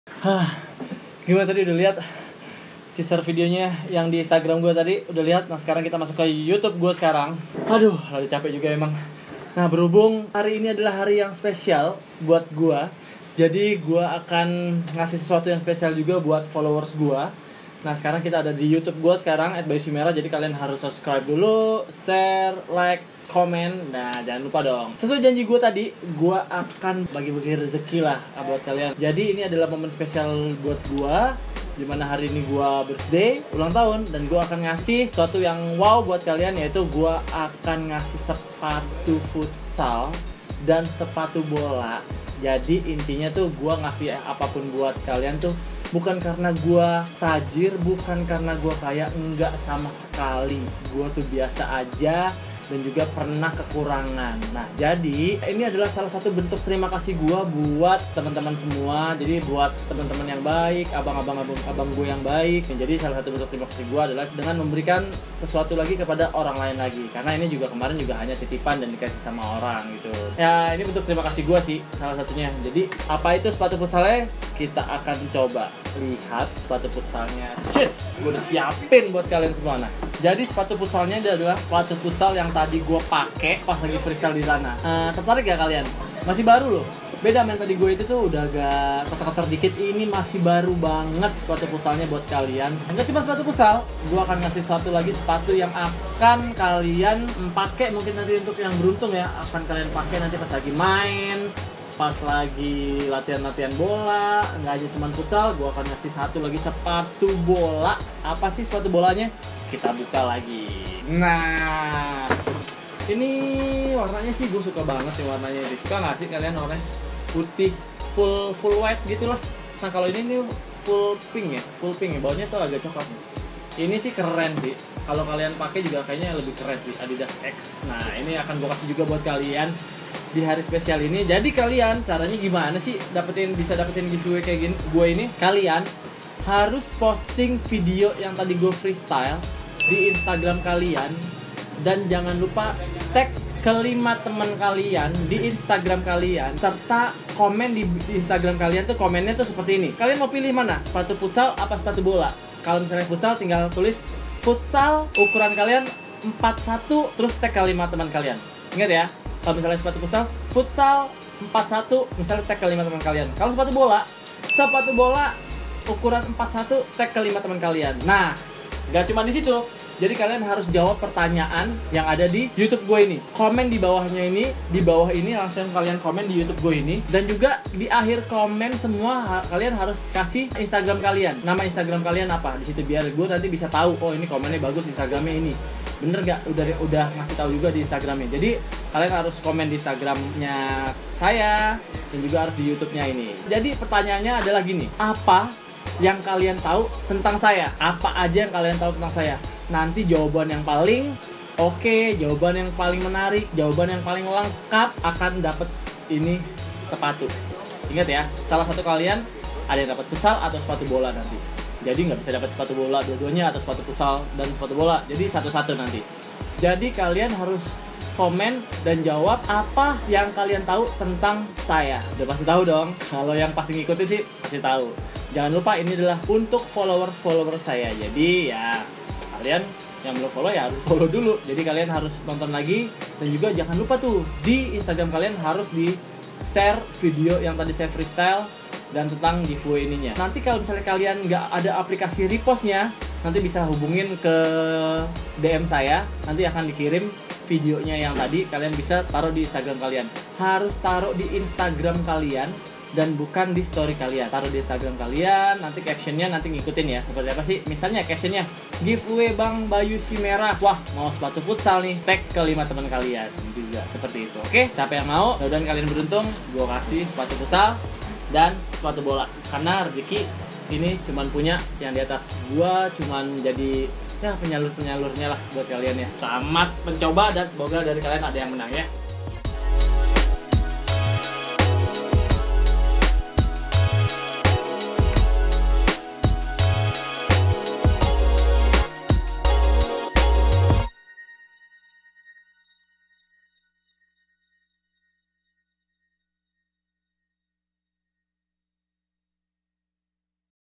GIVEAWAY 2 SEPATU ADIDAS SPESIAL ULANG TAHUN!!!
Hai Ballers

Kali ini gue akan giveaway dua sepatu dari Adidas, yaitu sepatu futsal dan sepatu bola.

Semoga Beruntung Ballers!!!